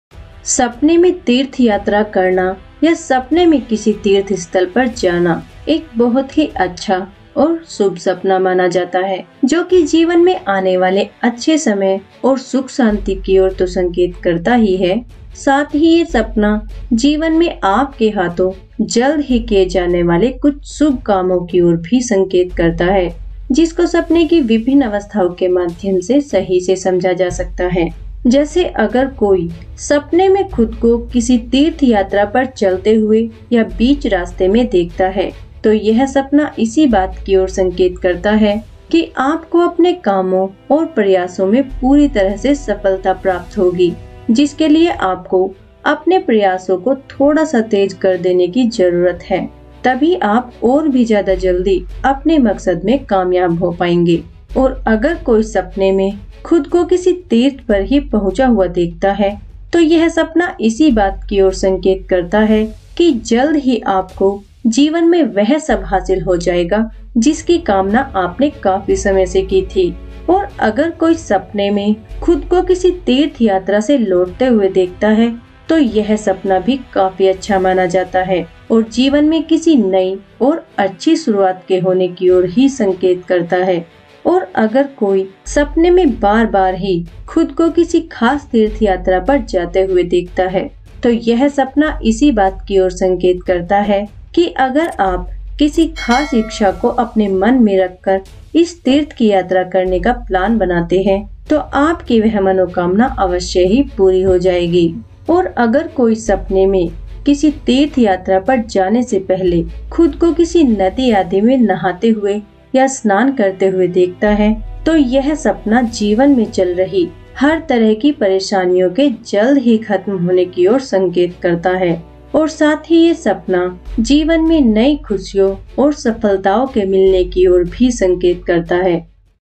Sapne me Tirth Yatra per Jana | Sapne me Thirth Yatra karna ya Dekhna
Sapne mein tirth yatra karna ya dekhna  kitna shubh ya ashubh hota hai.